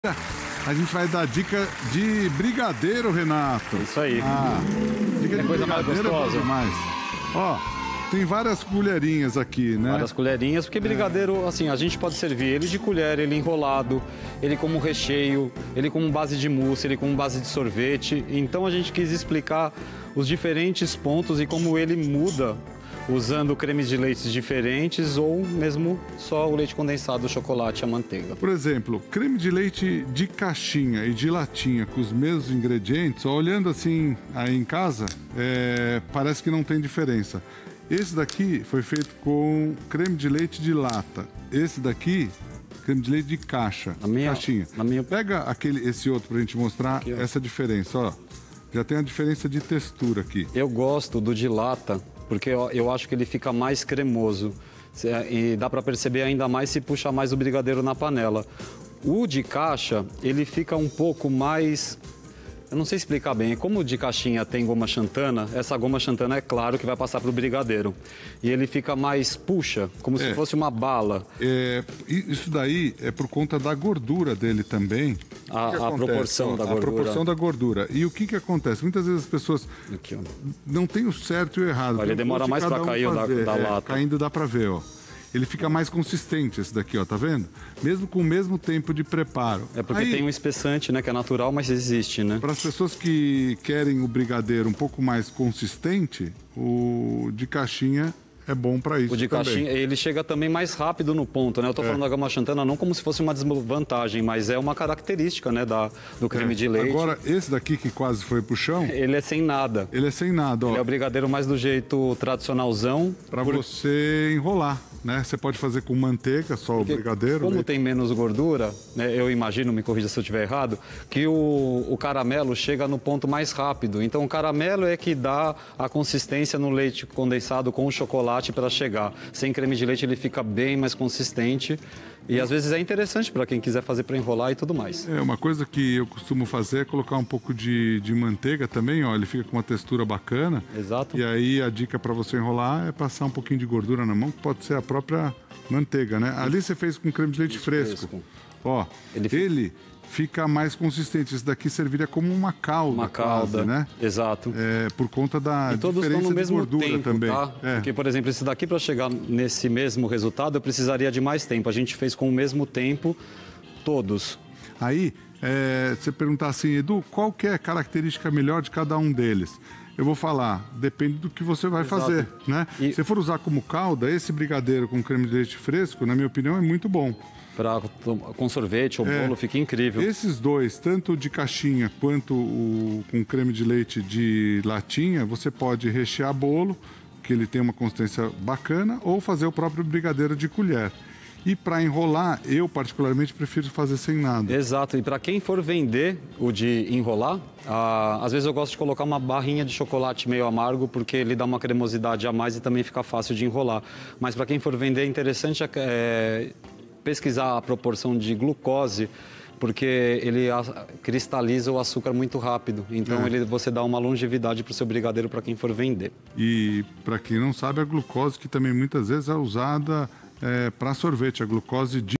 Qual é a função do creme de leite no brigadeiro? Edu Guedes explica | Band Receitas
Qual é a função do creme de leite no brigadeiro? O tipo de creme de leite que você usa na receita faz toda a diferença no doce. Aprenda truque do chef Edu Guedes! brigadeiro thechefcomedu BandReceitas 👏